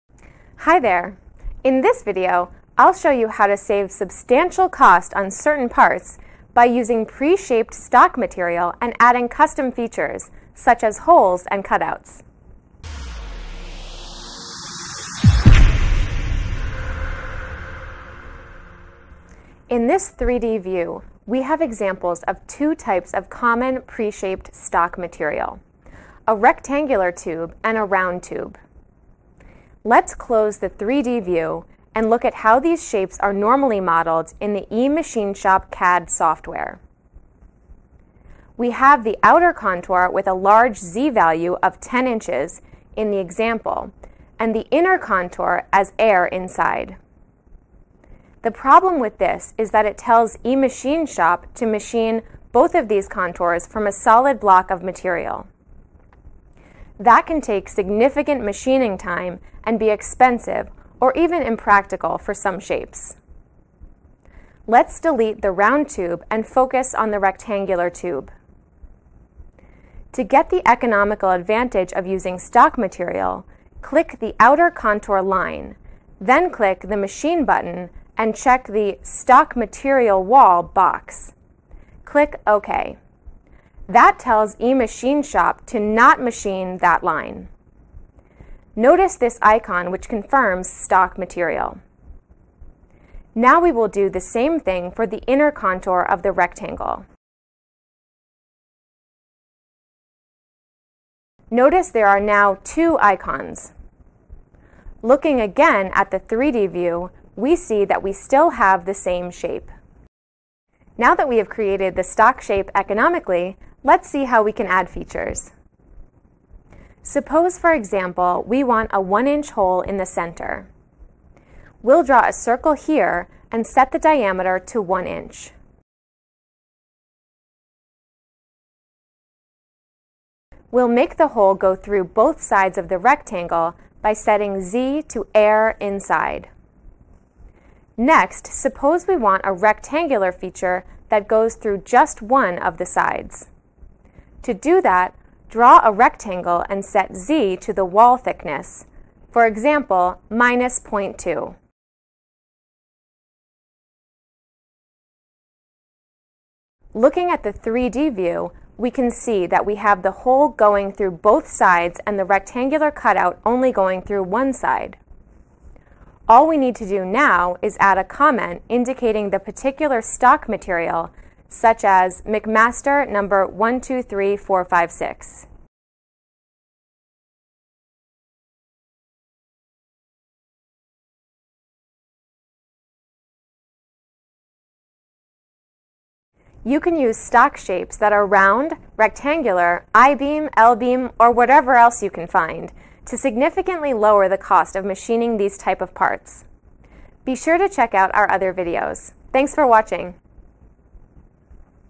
Pre Shaped Stock Tubes
For more information on Pre Shaped Stock Tubes, visit this

Hi there! In this video, I'll show you how to save substantial cost on certain parts by using pre-shaped stock material and adding custom features such as holes and cutouts.
In this 3D view, we have examples of two types of common preshaped stock material - a rectangular tube and a round tube.  Let's close the 3D view and look at how these shapes are normally modeled in the eMachineShop CAD software. We have the outer contour with a large Z value of 10 inches in the example and the inner contour as air inside. The problem with this is that it tells eMachineShop to machine both of those contours from a solid block of material. That can take significant machining time and be expensive or even impractical for some shapes.  Let's delete the round tube and focus on the rectangular tube. To get the economical advantage of using stock material, click the outer contour line, then click the machine button and check the "Stock material wall" box. Click OK. That tells eMachineShop to NOT machine that line. Notice this icon which confirms stock material. Now we will do the same thing for the inner contour of the rectangle. Notice there are now two icons. Looking again at the 3D view we see that we still have the same shape.

Now that we have created the stock shape economically, let's see how we can add features.
Let’s change to the Front view. Suppose for example, we want a half inch hole in the center here. We'll draw a circle and set the diameter to 0.5 inch. We'll make the hole go through both sides of the rectangle by setting Z to Air Inside.  Next, suppose we want a rectangular feature that goes through just one of the sides. To do that, draw a rectangle and set Z to the wall thickness - for example minus point two.  Looking at the 3D view, we can see that we have the hole going through both sides and the rectangular cutout going through only one side. All we need to do now is add a comment indicating the particular stock material such as mcmaster #123456.

You can use stock shapes that are round, rectangular, I-beam, L-beam, or whatever else you can find to significantly lower the cost of machining these types of parts.